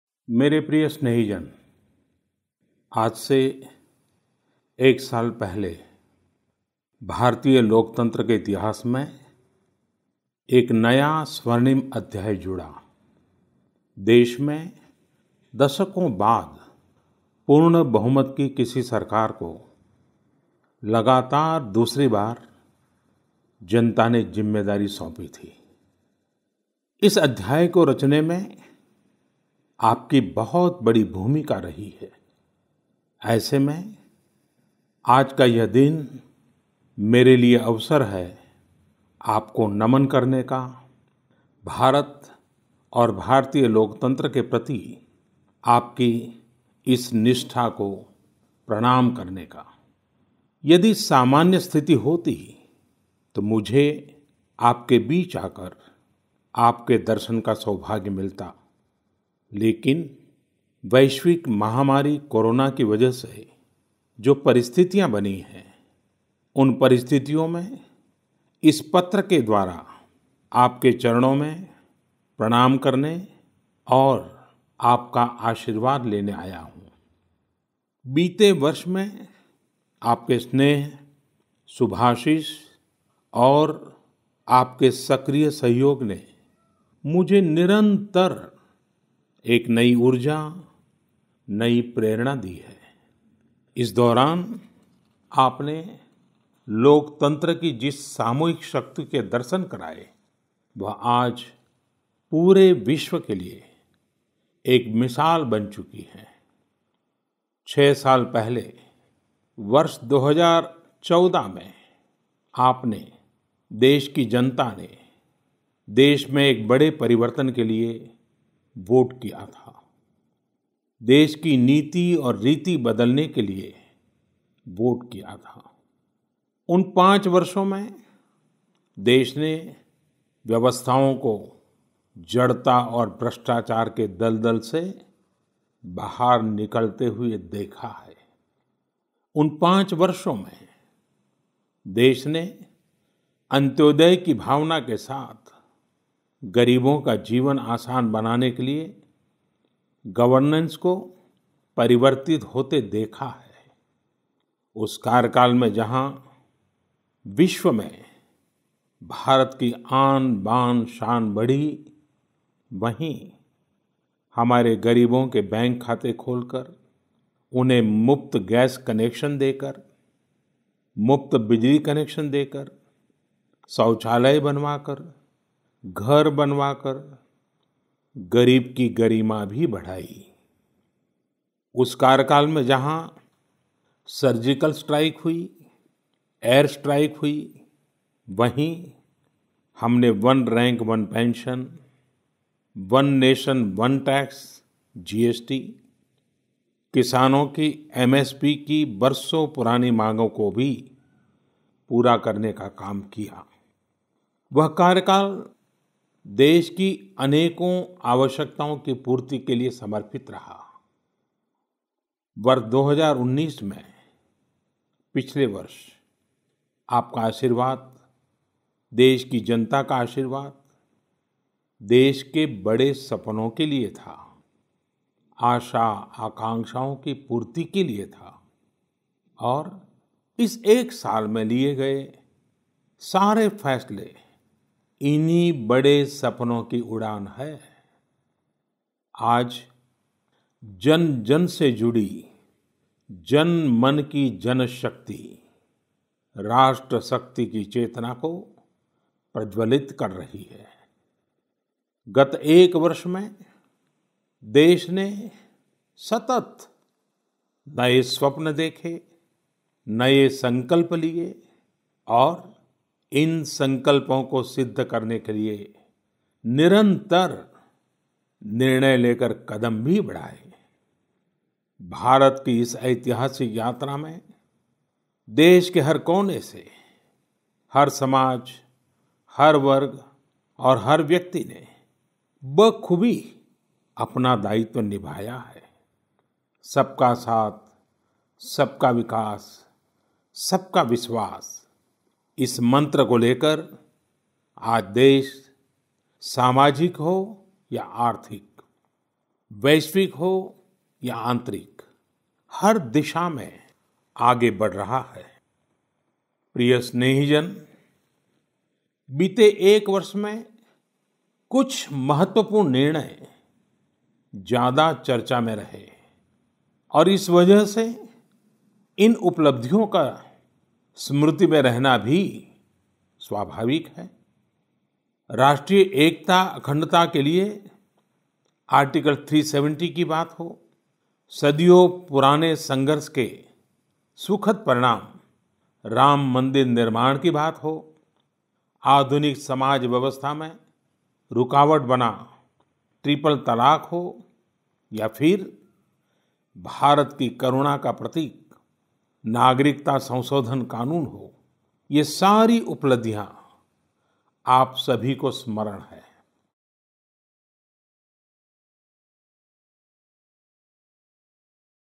PM Modi recounts ‘Article 370’, ‘Surgical strike’ on 1st anniversary of 2nd term
Prime Minister Narendra Modi wrote a letter to the citizens of the nation on the first anniversary of his second term. He listed abrogation of Article 370, settlement of the Ram Temple issue, criminalisation of Triple Talaq and the amendment to the Citizenship Act among the key achievements during his second term.